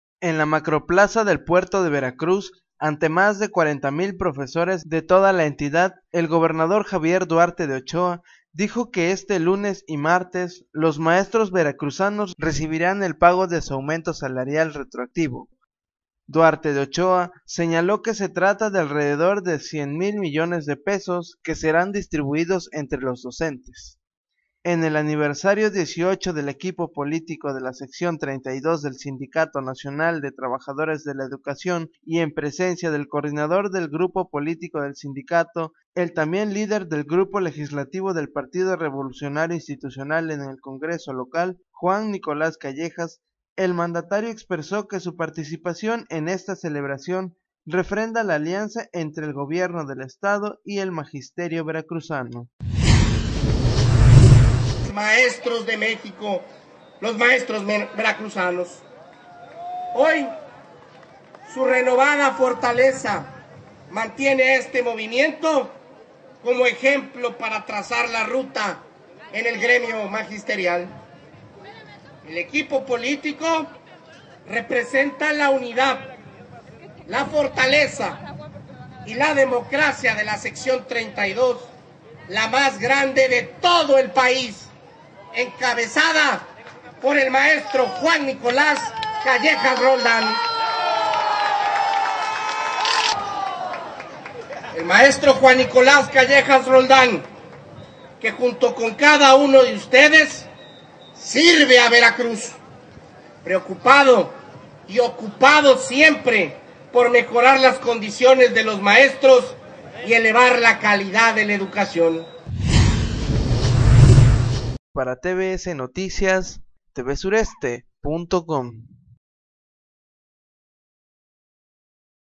TVS Noticias.- Javier Duarte Se Reúne Con Profesores de Todo El Estado Durante el Aniv del SNTE
- Javier Duarte Se Reúne Con Profesores de Todo El Estado Durante el Aniversario del SNTE